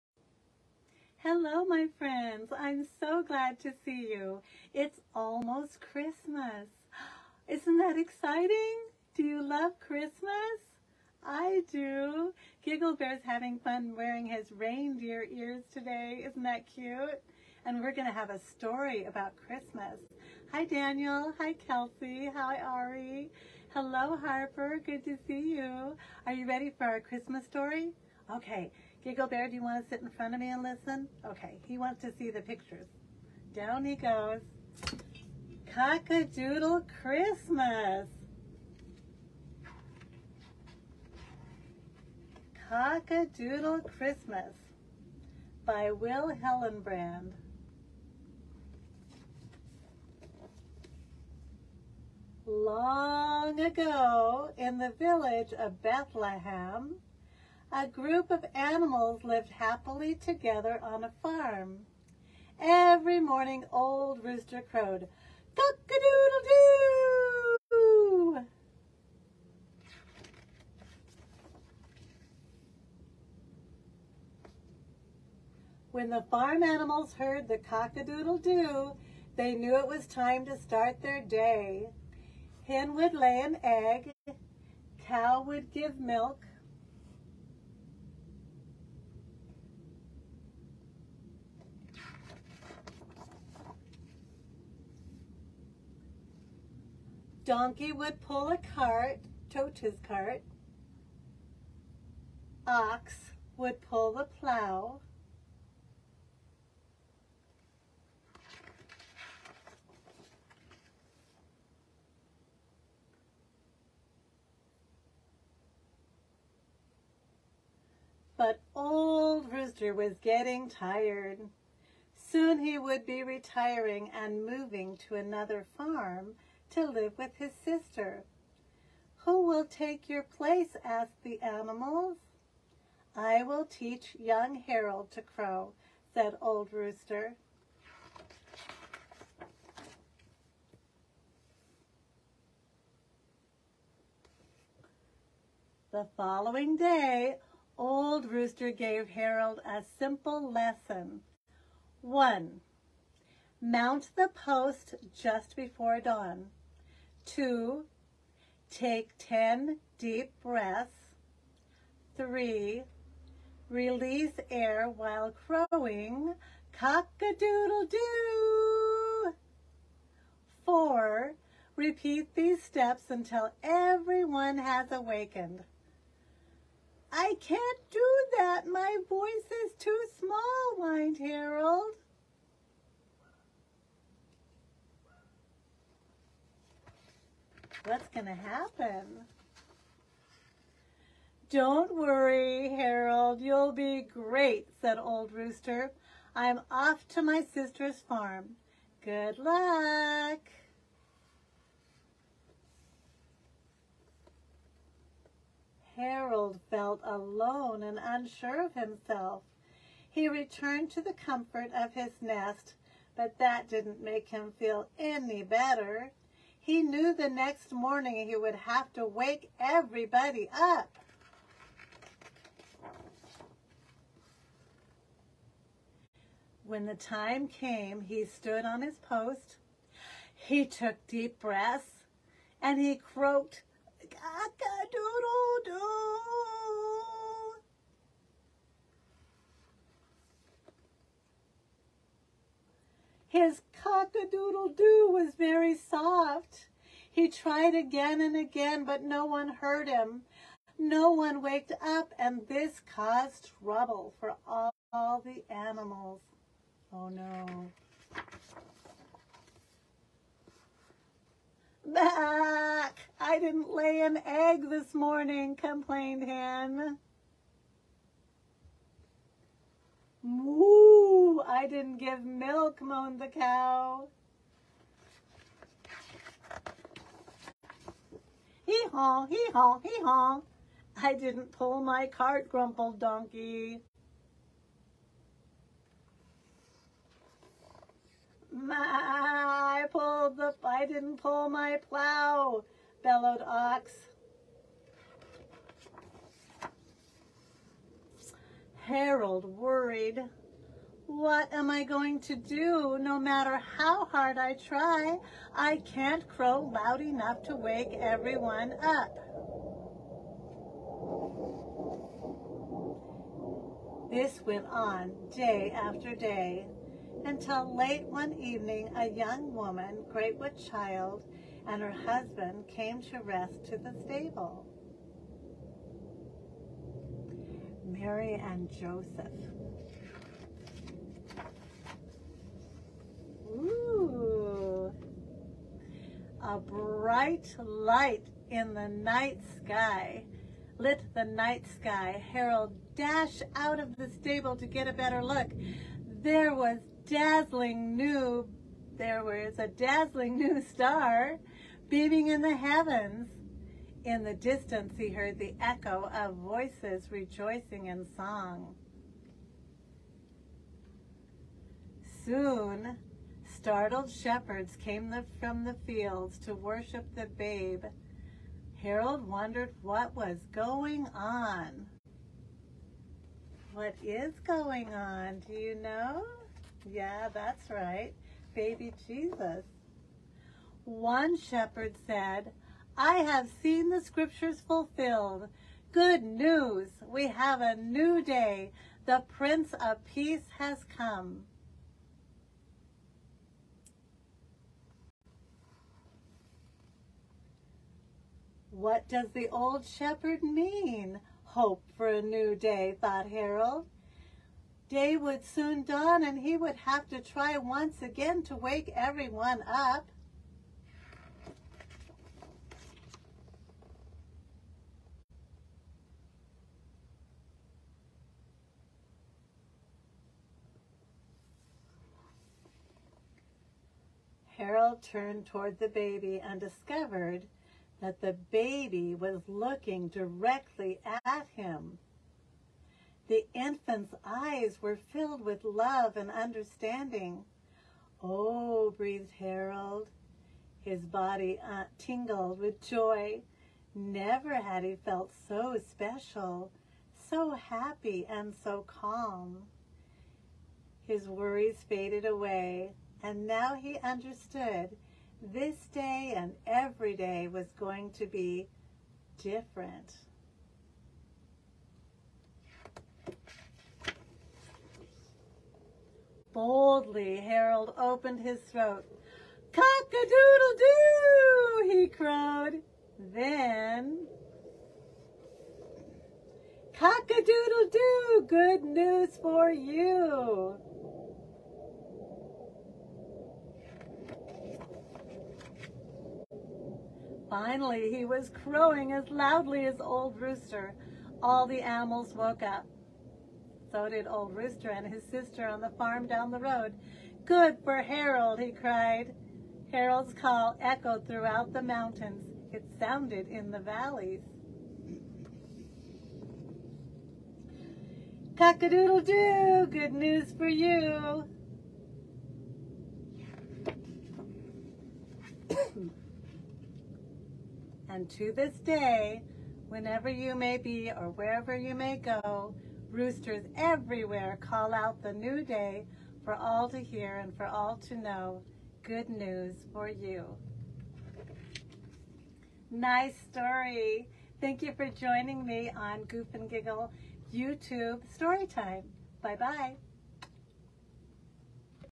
Cock-a-Doodle Christmas
When Old Rooster retires and leaves the farm, young Harold takes up the job of waking the farm animals each morning. But, Harold has a soft voice until the sky lights up and a miraculous birth takes place. Harold shouts the Good News.